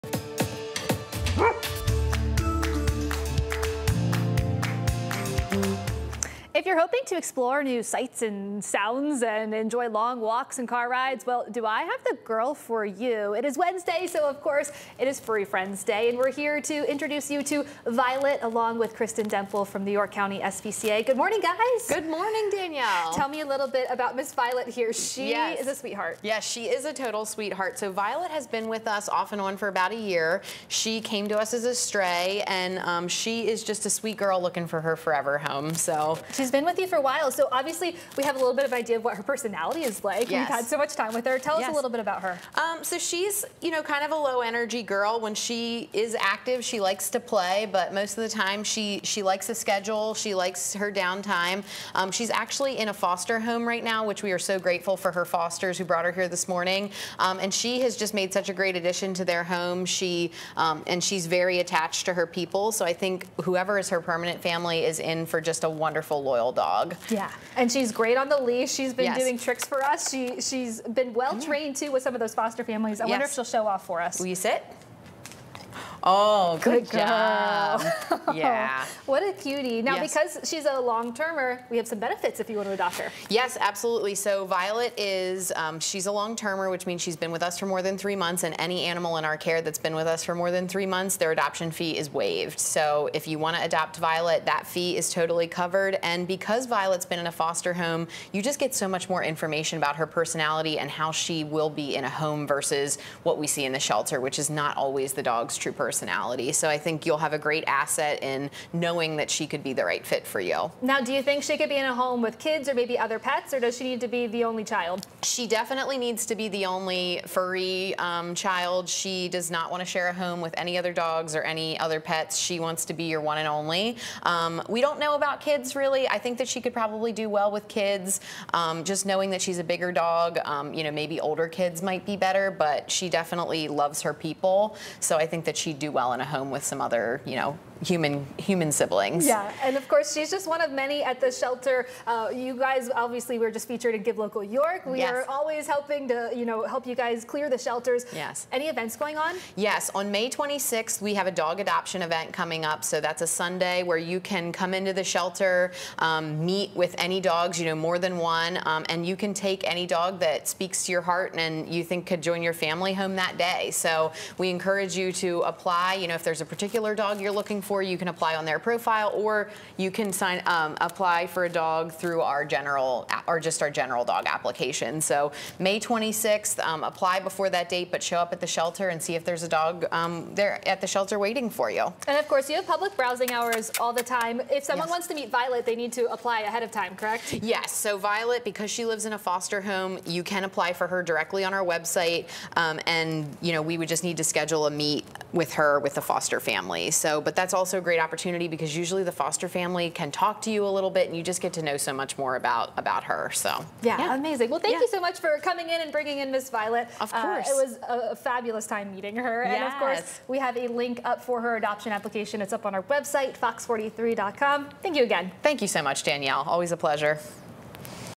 Pet on the Set | Furry Friends with Violet, the dog!
This week's Furry Friend is Violet, brought to us by the York County SPCA.

Violet is estimated to be 5 years old and is a bully mix. She has been at the York County SPCA for nearly a year. And while she's currently living with a foster family, she's looking to find her permanent place in the world.

Her foster family says that Violet loves to go on car rides and long walks. She enjoys to explore new sights and smells. Violet has excellent leash skills and is very obedient too.